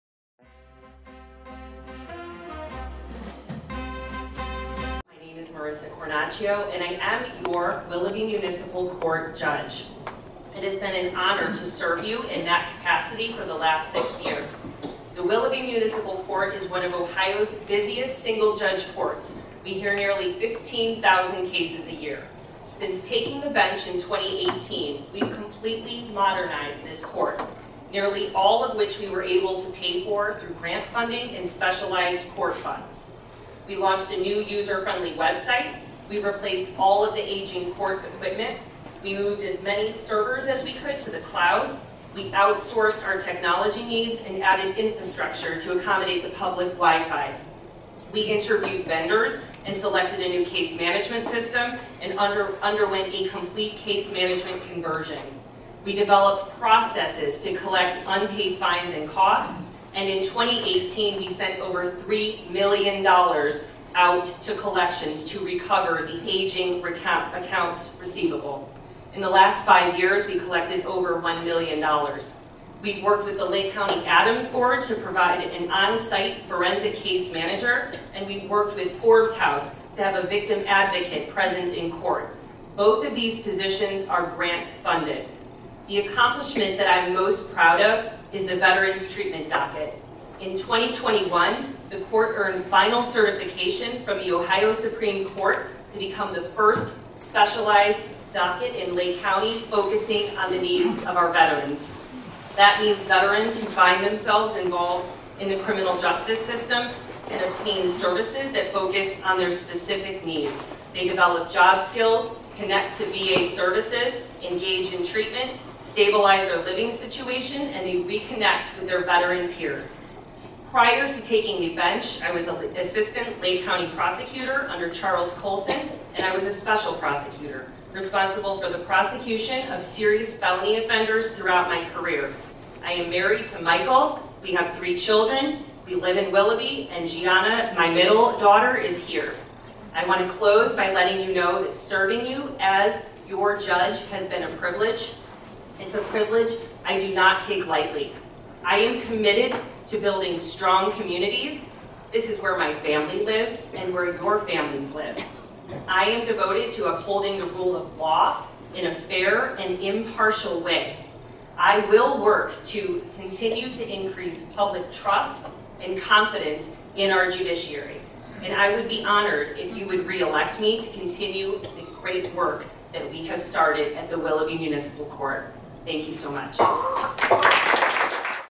Judge Marisa L. Cornachio at the League of Women Voters Candidate Forum
Incumbent Willoughby Municipal Judge Marisa Cornachio states her case for re-election.